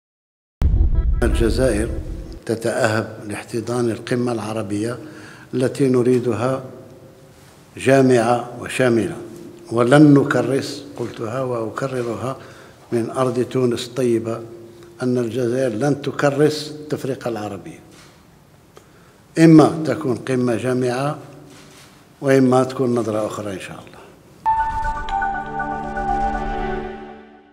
الرئيس تبون: الجزائر لن تكرس التفرقة العربية وتريد قمة عربية جامعة وشاملة
تونس (واج)- أكد رئيس الجمهورية عبد المجيد تبون خلال ندوة صحفية مشتركة مع الرئيس التونسي قيس سعيد  على هامش زيارته لتونس أن الجزائر تسعى إلى عقد قمة عربية "جامعة وشاملة" وأنها "لن تكرس التفرقة العربية".